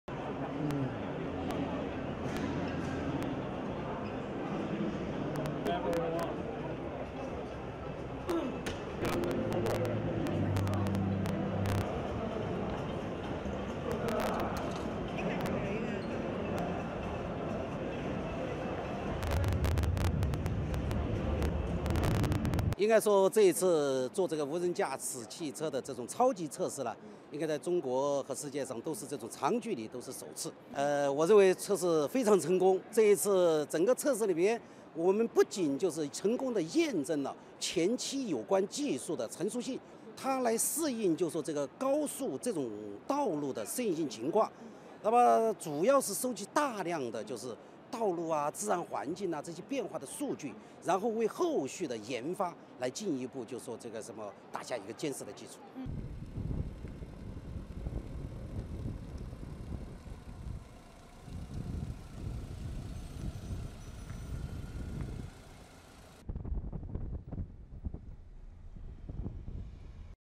Self-driving Cars Highlighted at Auto China 2016
Self-driving cars were highlighted at Auto China 2016, which kicked off in Beijing on Monday.

Two self-driving cars developed by Chang'an Automobile Group drew the attention of car fans because they drove about 2,000 kilometers from southwest China's Chongqing to Beijing for the exhibition.

It marked the first time that China's self-driving cars conducted a long-distance test and it also represents the latest development of China's self-owned brand automobiles in this field.

"It is the first time for a self-driving car to conduct such a long-distance test. I think the test is very successful. We have proved that the technology is mature enough and can adjust to different road conditions. Meanwhile, we have also collected lots of data on road conditions and natural environment to lay a solid foundation for further research and development," said Zhu Ronghua, president of Chang'an Automobile Co., Ltd.